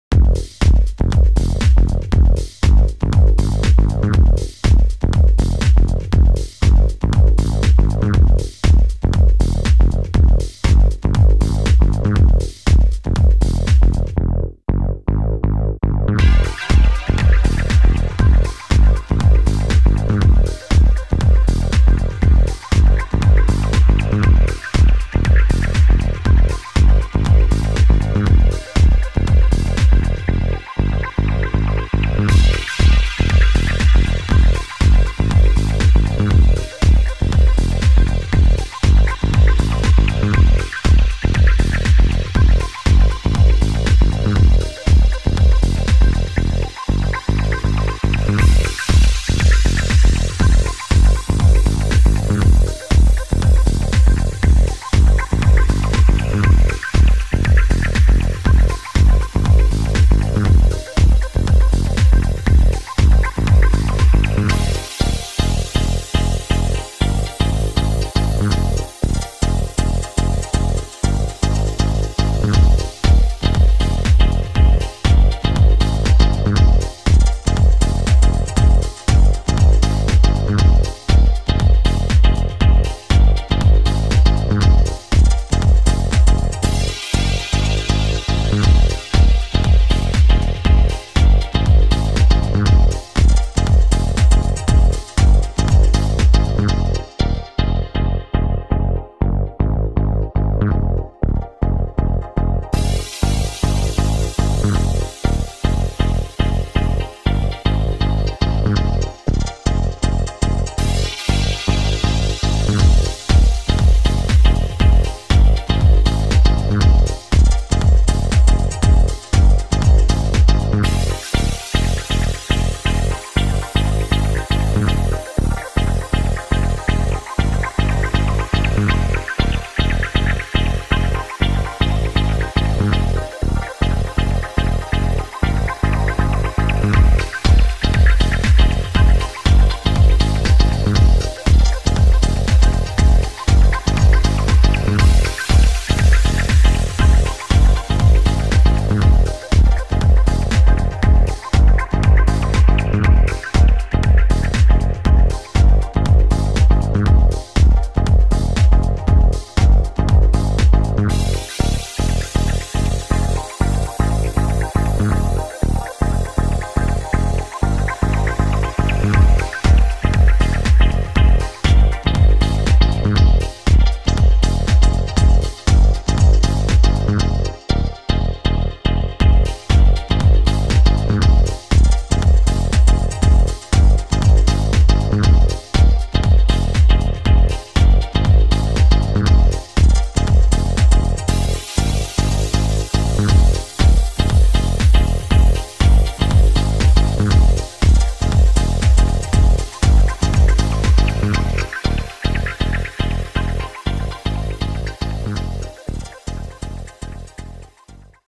Photo Shooting in Moscow Backstage Making Of
Model Veronica April 2012 Making Of Photo Shooting